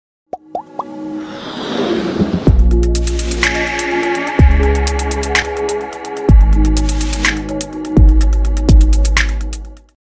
Bbu craic intro trailer | for more stay tuned ! Subscribe us
Intro to my you tube channel.
In next video I will show you how to create YouTube intros yourself for your own channel using blender , Adobe effects or song Vega pro